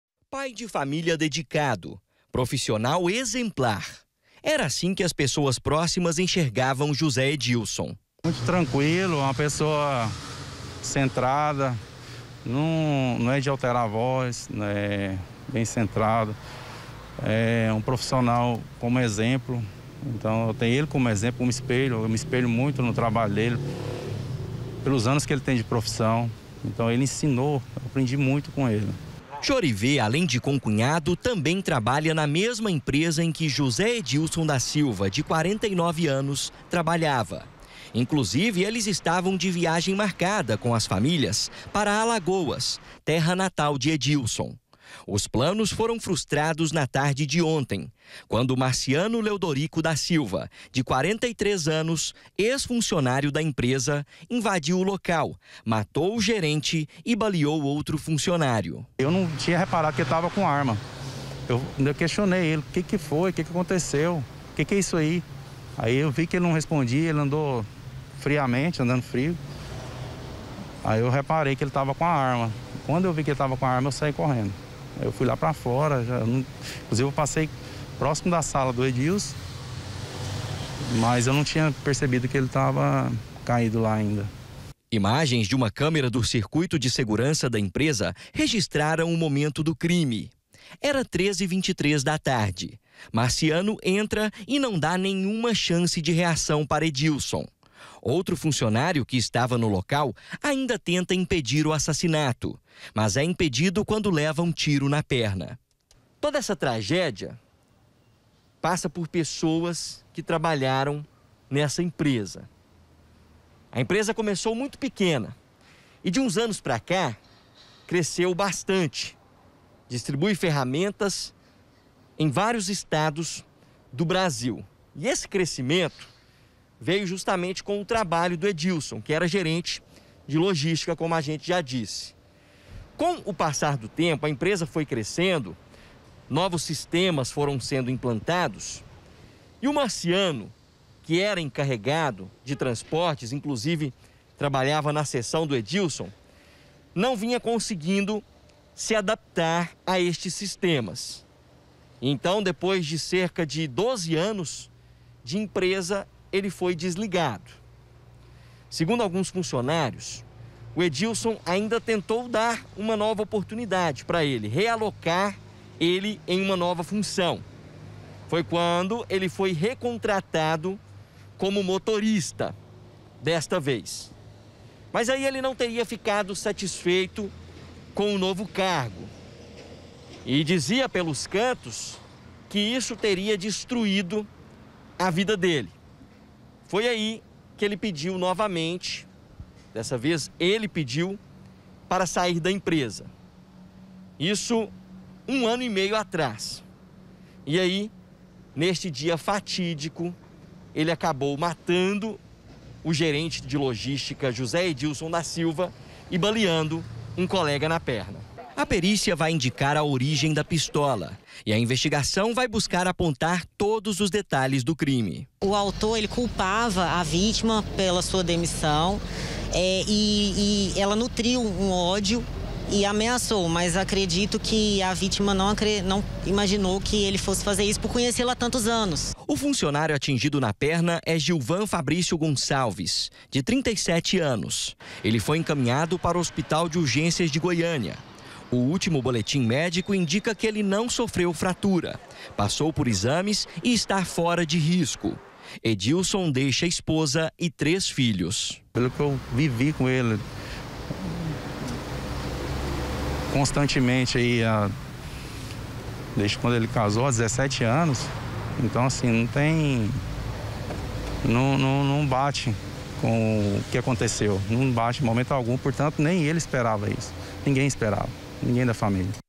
CRIME BÁRBARO: EX-FUNCIONÁRIO MATA GERENTE DE LOJA
COLEGAS DE TRABALHO DO GERENTE ASSINADO POR EX-FUNCIONÁRIO DE UMA FERRAGISTA, LAMENTAM A MORTE DO COLEGA. EDILSON ERA VISTO POR TODOS COMO UMA PESSOA TRANQUILA E QUE GOSTAVA DE AJUDAR O PRÓXIMO. UMA PERÍCIA DA POLÍCIA CIVIL DEVE INDICAR ONDE E COMO O AUTOR DO ASSASSINATO CONSEGUIU A ARMA DO CRIME.